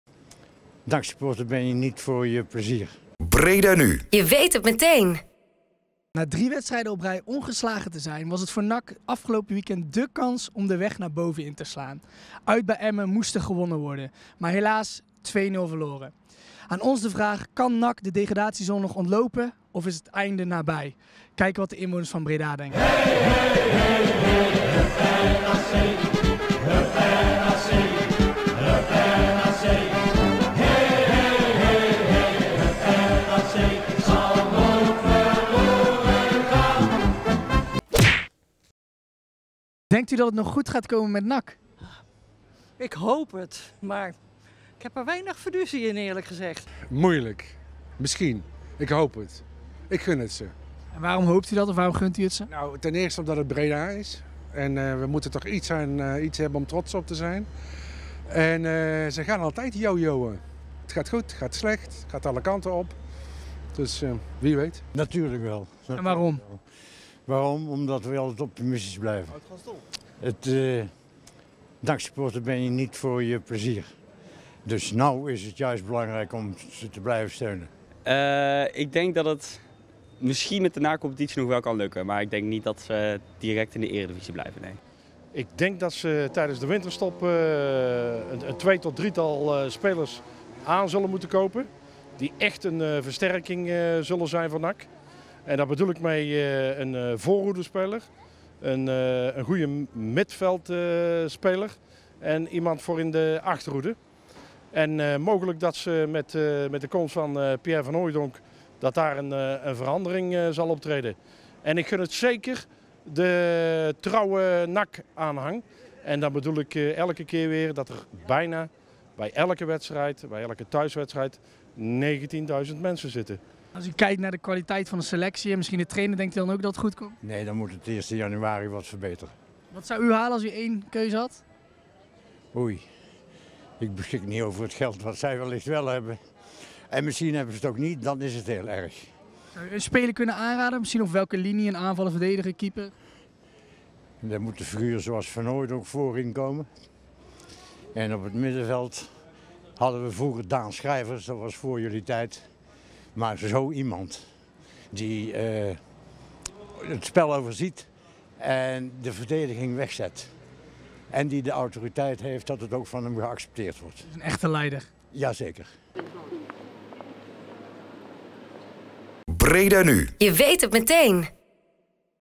Vraag op Straat - NAC
Gaat NAC het nog redden dit seizoen na de nederlaag tegen Emmen?